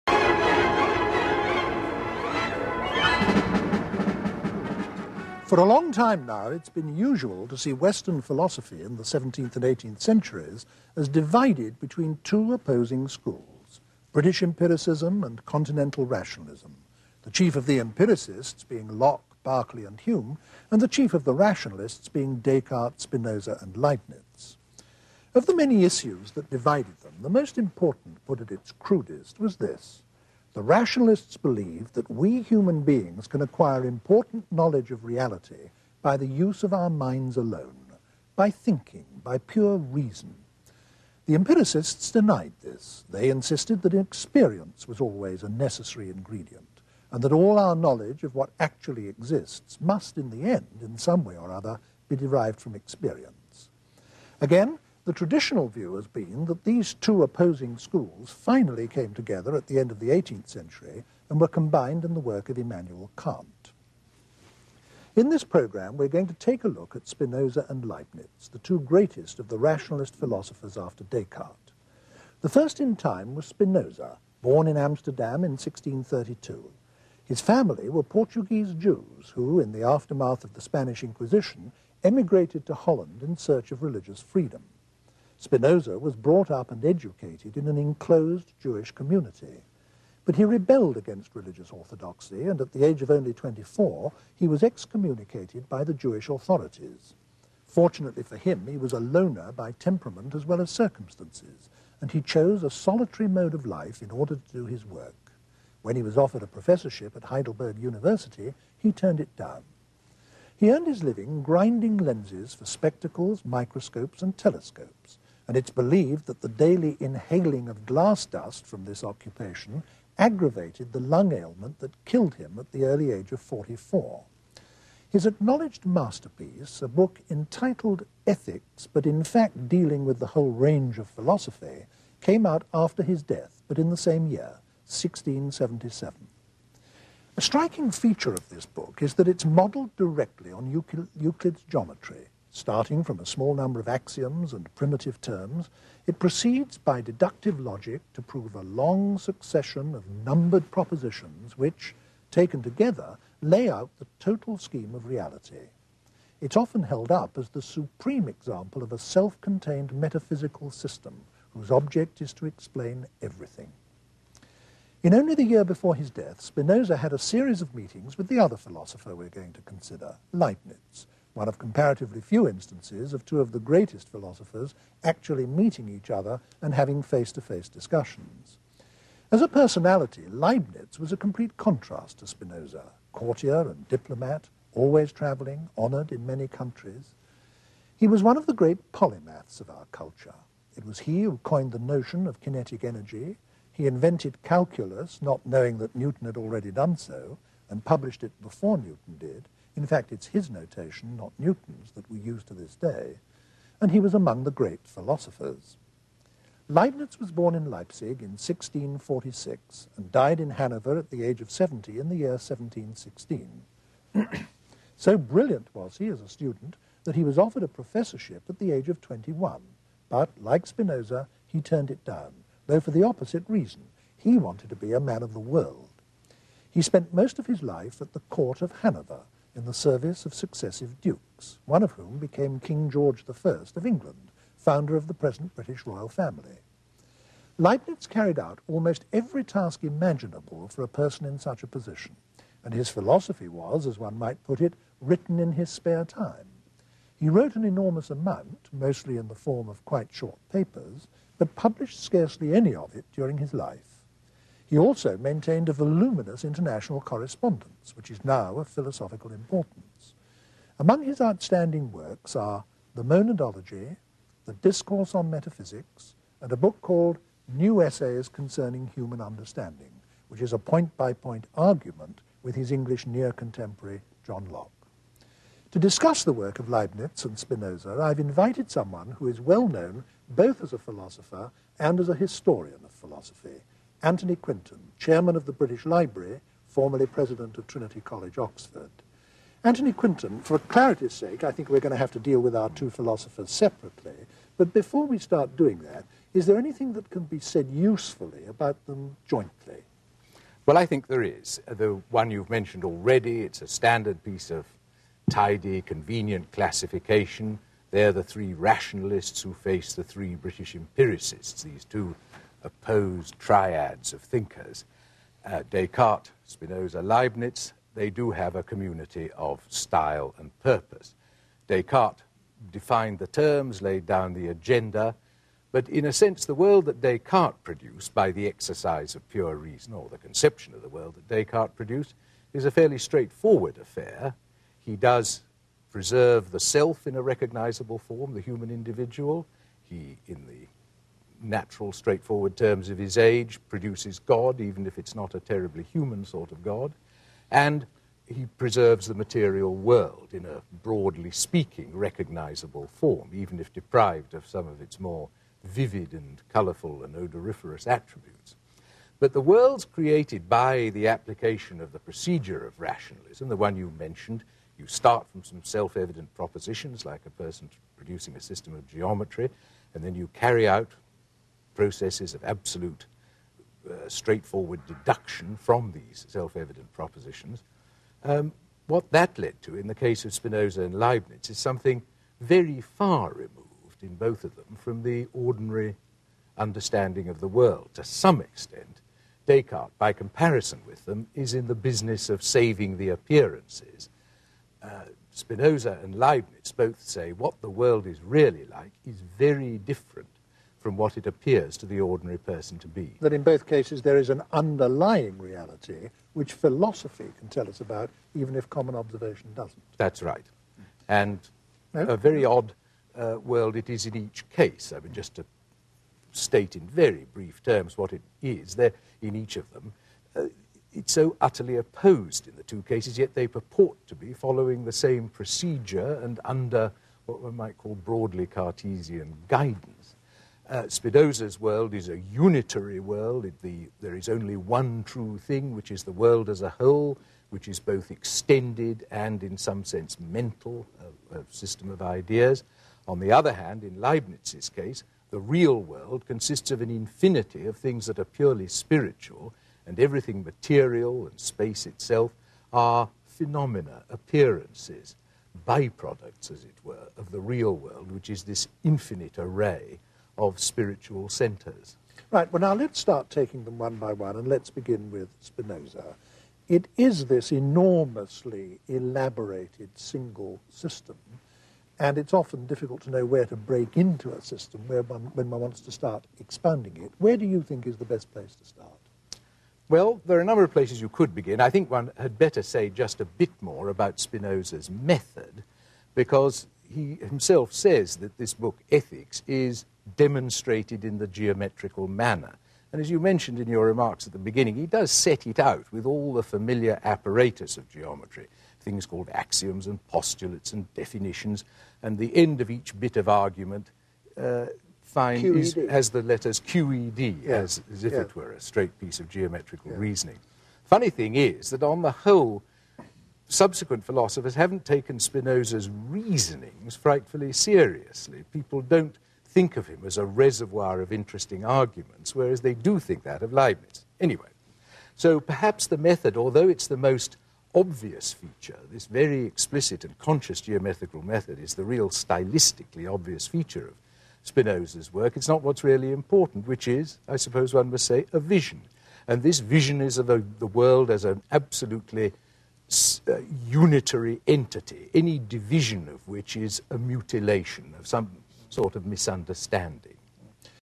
Anthony Quinton on Spinoza and Leibniz: Section 1
The ideas of rationalist philosophers Baruch Spinoza and Gottfried Leibniz are examined in this program by philosophy Anthony Quinton. Spinoza favors a pantheistic God who has matter and mind as two attributes, and who is the ultimate substance and explanation of the world. Leibniz sees the real world as consisting of an infinity of things purely spiritual, where everything, including space, is a phenomenon—a by-product of areal world with an infinite array of spiritual centers. Both philosophers construct a world that is very different form what the average person perceives, and both reject Cartesian duality.